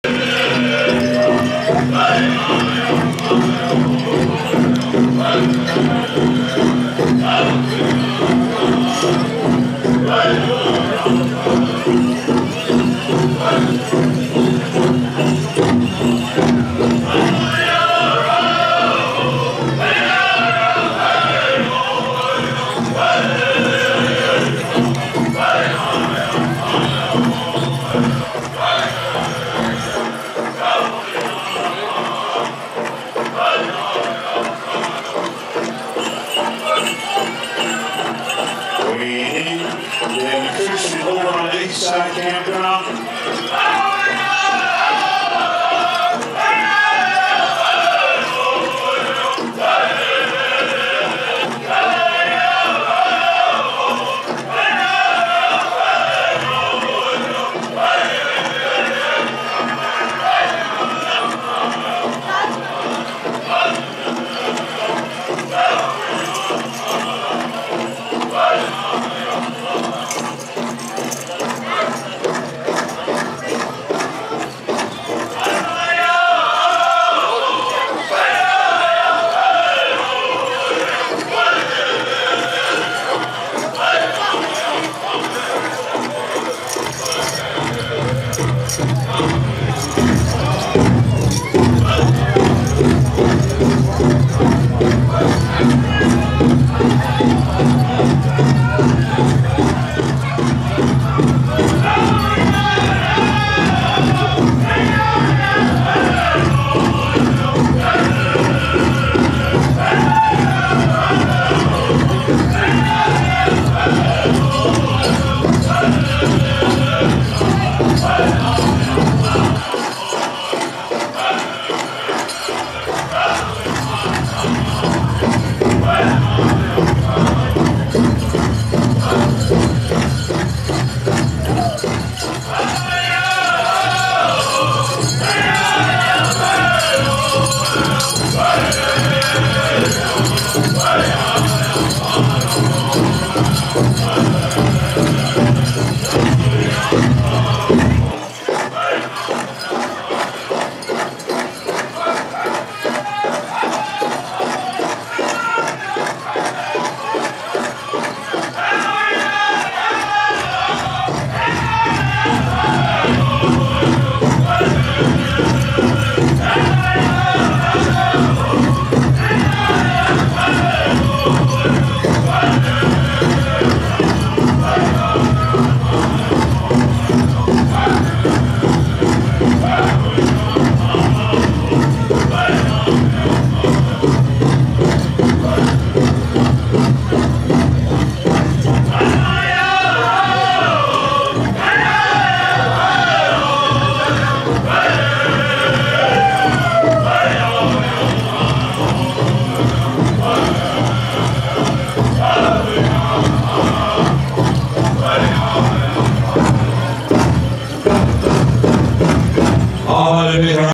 Teen Girls Traditional in Sisseton July 2018
Sunday Finals at Sisseton Powwow July 6 - 8, 2018 in Sisseton, South Dakota